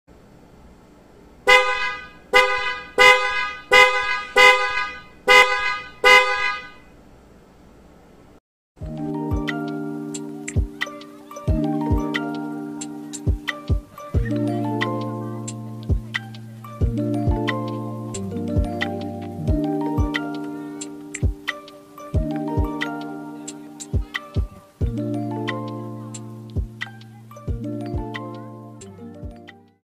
Car Horn Snail Bm Horn
SNAIL HORN

warranty 3month

Descriptions:
Omni Beyond Horn Free Relay
-12 Voltage
-400 Hz(High) / 300 HZ (Low) sound frequency
- 100mm diameter

Features:
1. Universal Fit
2. Easy to Install
3. Durable & Price Friendly
4. Protection of Speaker Use Improved Effectively
5. Fits With 12V Power Supply
6. Waterproof Technology

*WARNING: STRONG VOICE, HARSH ENVIRONMENT FORCEFUL SPEAKERS VOICE CLEAR*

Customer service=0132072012
WeChat=ho_0542